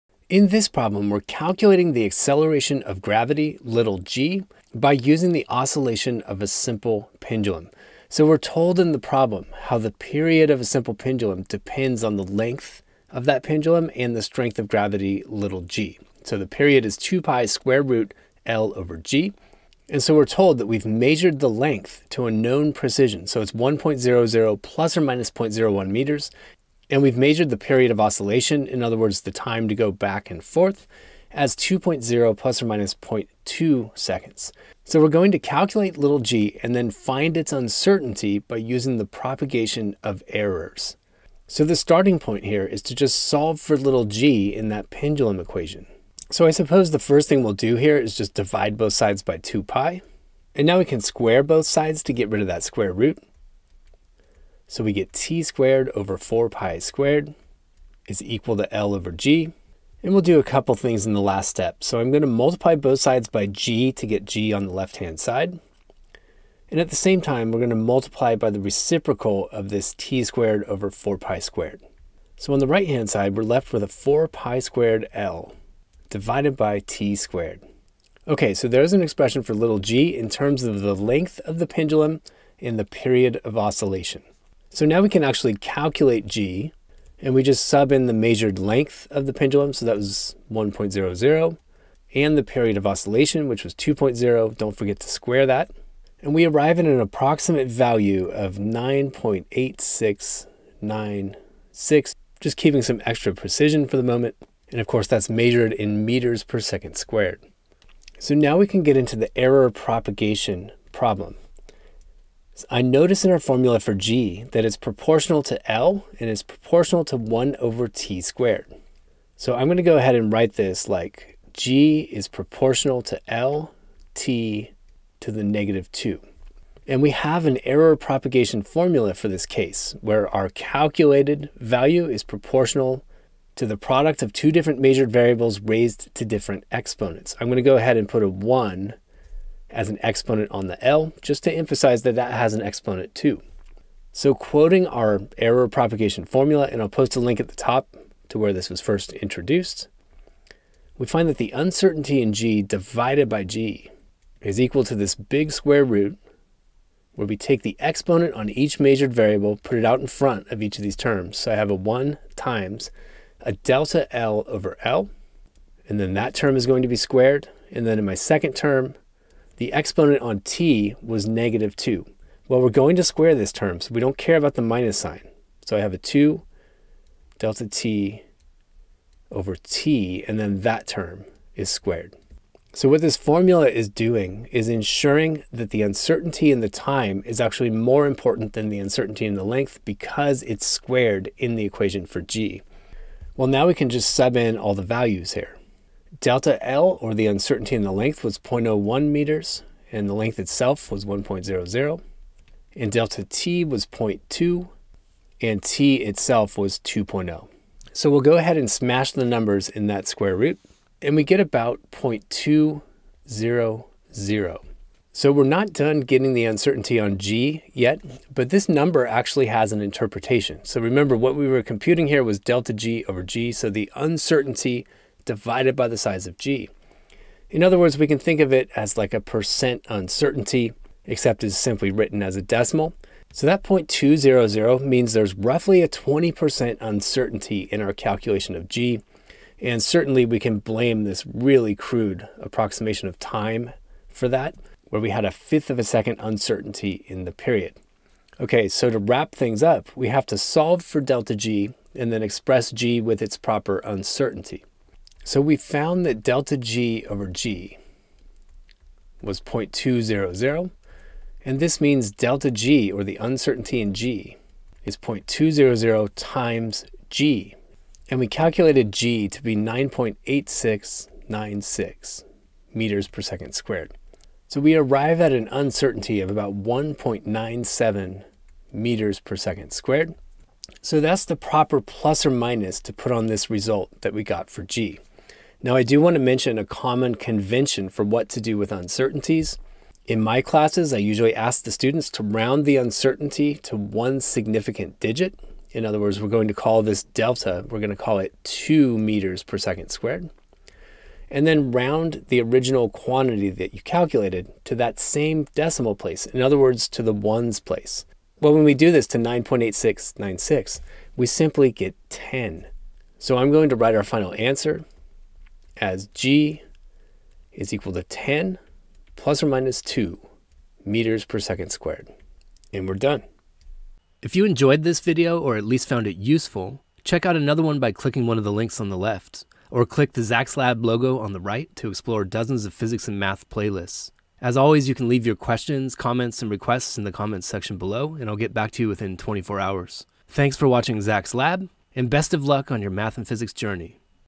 Using a pendulum to measure gravity (including propagation of errors).
Given the length and period of a pendulum (with uncertainty), we calculate the acceleration of gravity using a simple pendulum AND we compute the uncertainty in g using propagation of errors.

Recall the formula for the period of a simple pendulum is T=2pi*sqrt(L/g).  We solve the period formula for g and get g=4pi^2*L/T^2.  This allows us to calculate the acceleration of gravity using the length and period of the simple pendulum.

Next, we need to calculate the uncertainty using propagation of errors.  Since g is proportional to L and T^-2, we can use our propagation of errors formula for a product of variables raised to a power.  We calculate the relative uncertainty in g, and that turns out to be about 0.2, or a 20% uncertainty due primarily to the huge uncertainty in T for this measurement.  Finally, we multiply by g, and using a pendulum to measure gravity, we have a final answer of 10+/-2 m/s^2, which captures the accepted value of about 9.8m/s^2 for the acceleration of gravity.